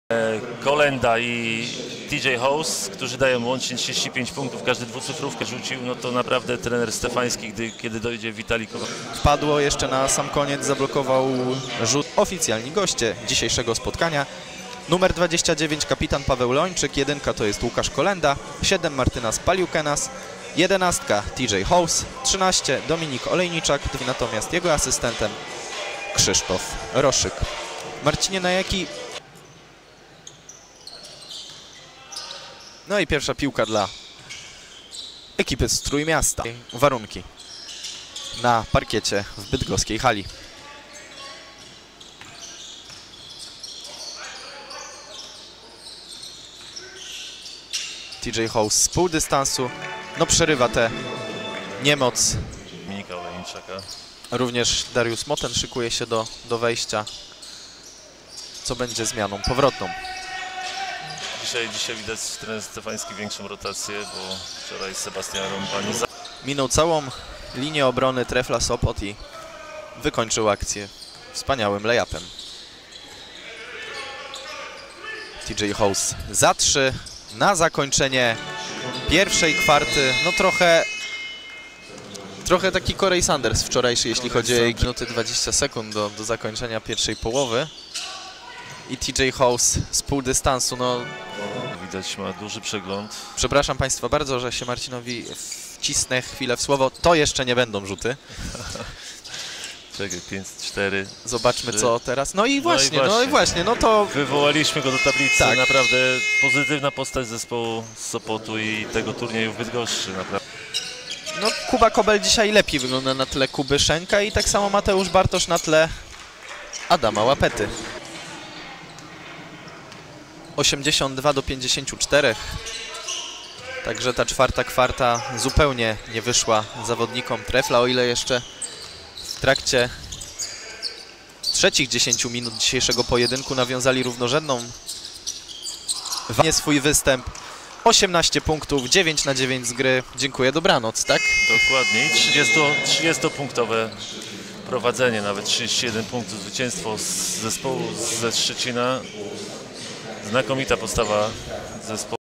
TJ Haws 17 Points on 7/8 FG in Poland (August 16, 2020)
TJ Haws scored a team high 17 points on 7/8 shooting as Trefl Sopot was blown out in the Abramczyk Cup. Haws also scored 11 points the day before in his debut.